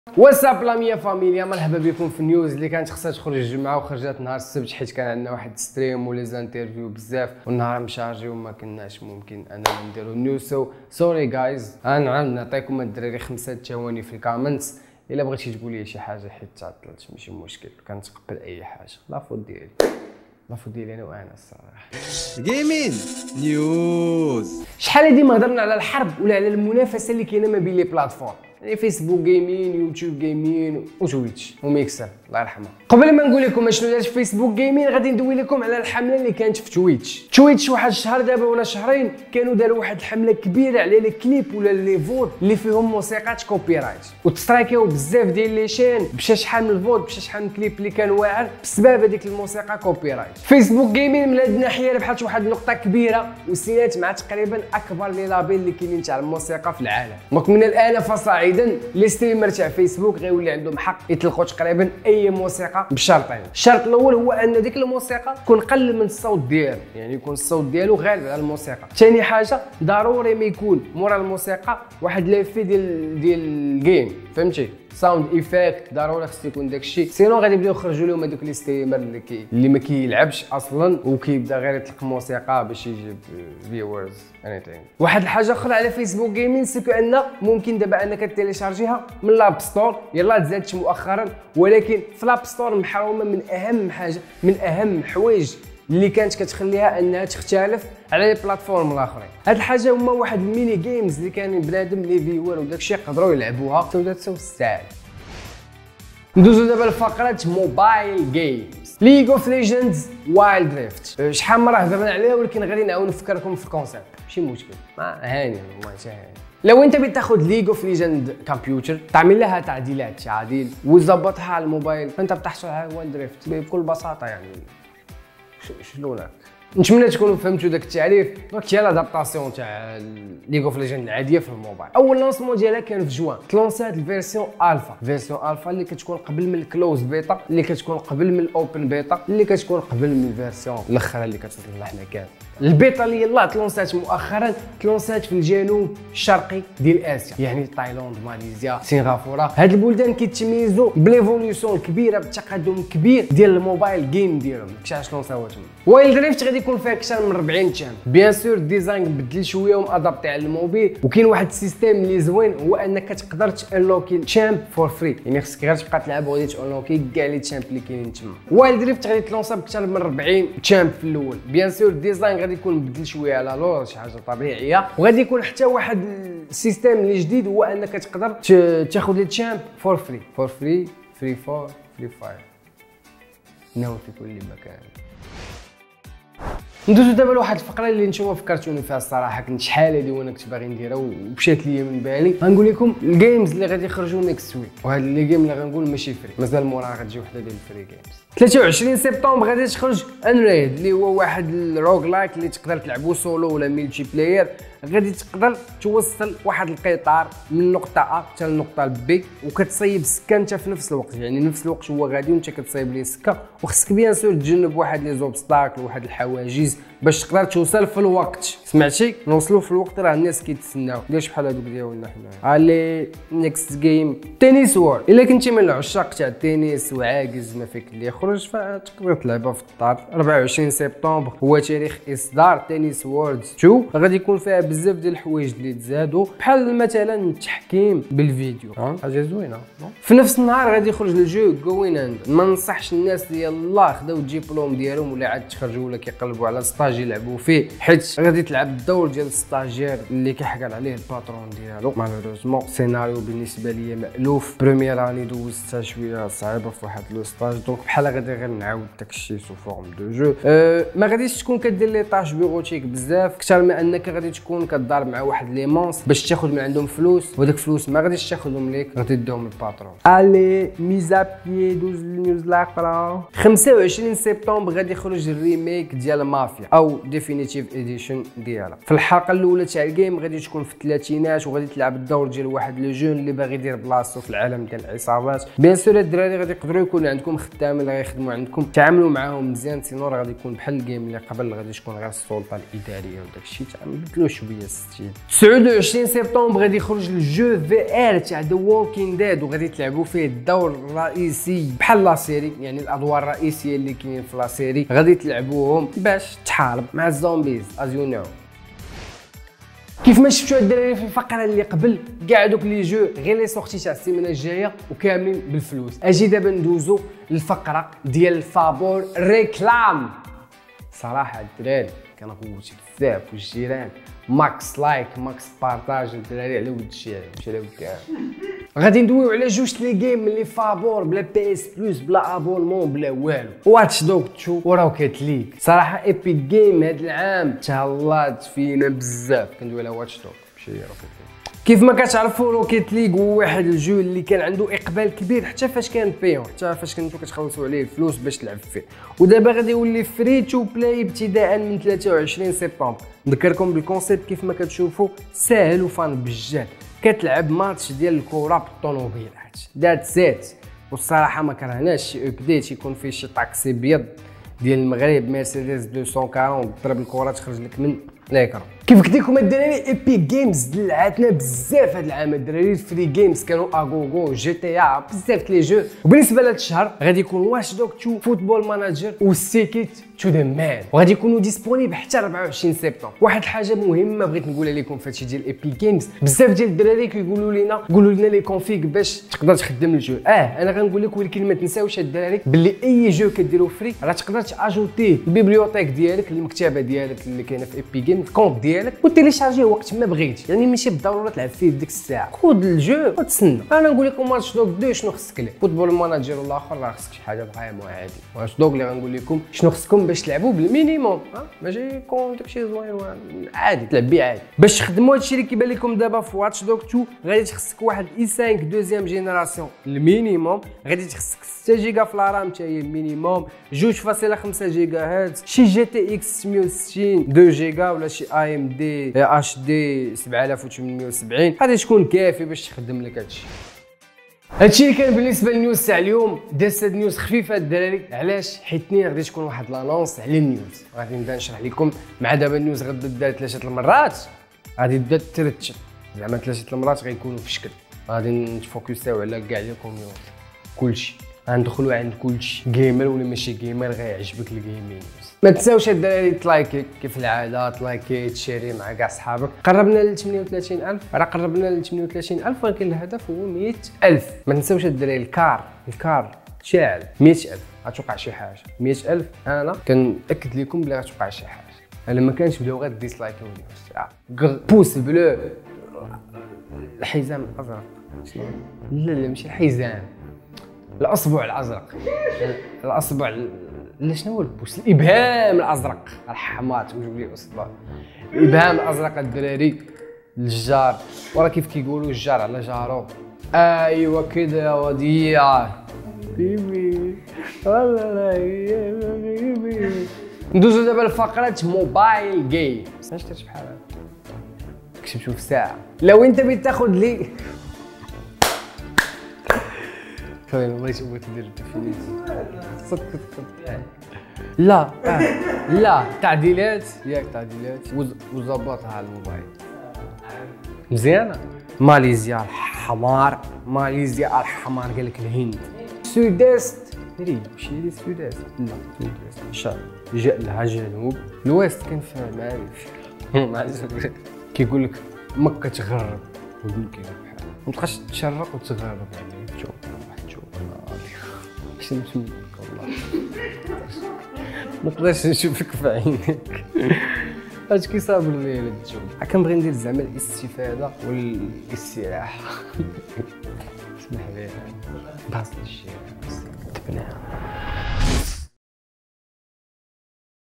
[Lgaming News] : WilD Rift - Free Games - Rocket League - Facebok Gaming - ألعاب مجانية ...
حدثنا يوسف في هذا الموجز الإخبار عن الألعاب المجانية و الألعاب التي سيتم إصدارها الأسبوع المقبل و أعطى معلومات عن اللعبة الجديدة لرايوت غايمز وايلد ريفت و تكلم عن فايسبوك غايمينغ الذي وقع مع أكبر شركات الموسيقى ، فرجة ممتعة

Intro: (0:00)
Trending: (0:24)
Mobile Games: (1:46)
Release dates: (3:06)
Free Games: (5:09)
Outro: (7:14)
Bêtisier: (7:54)